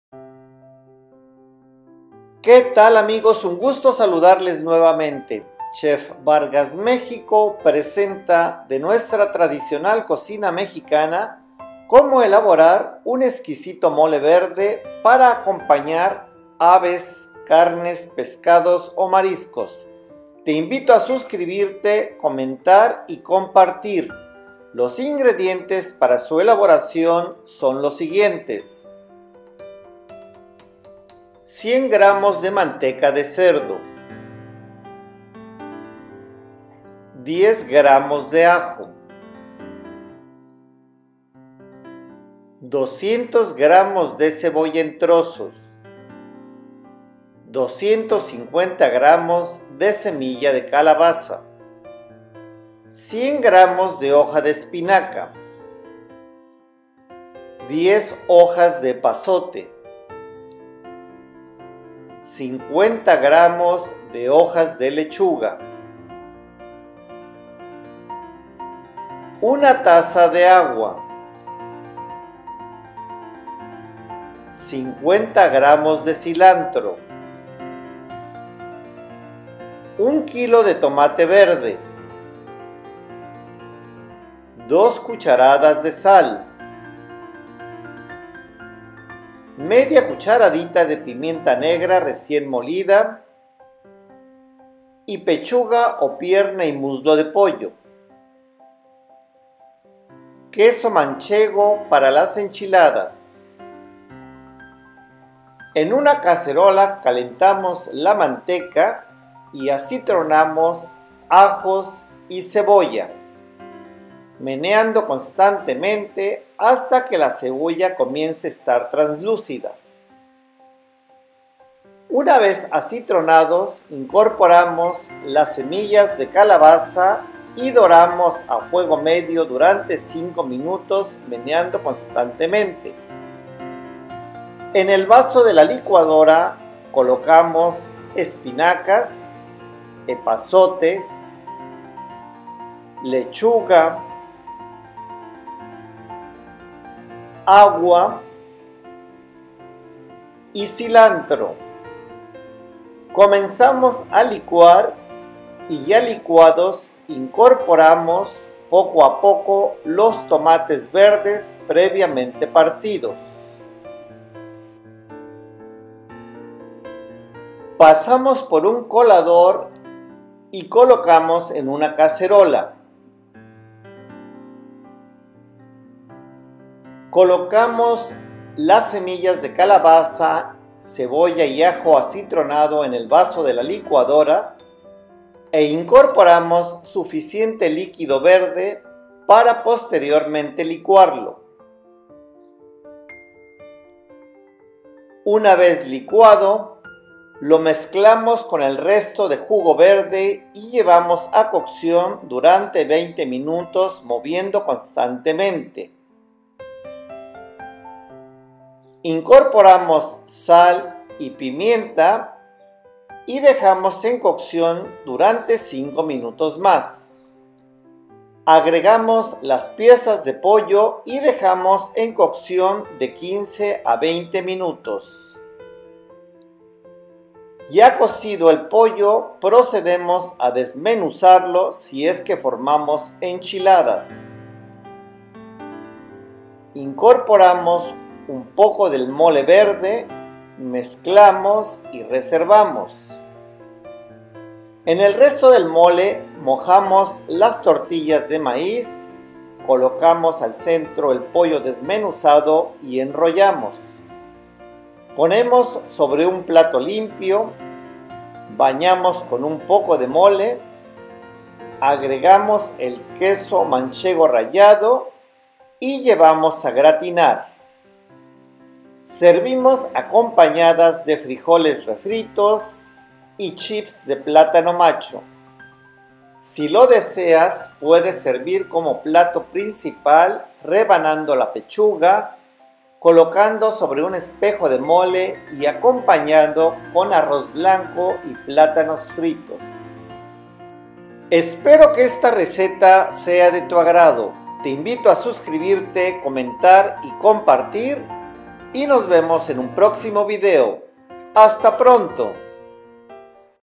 MOLE VERDE / GREEN MOLE
Con el que podrás acompañar aves, carnes, pescados o mariscos, si deseas que sea picante agrega un poco de chile serrano licuando junto con las hierbas.

Te invito a elaboralo, comentar y compartir.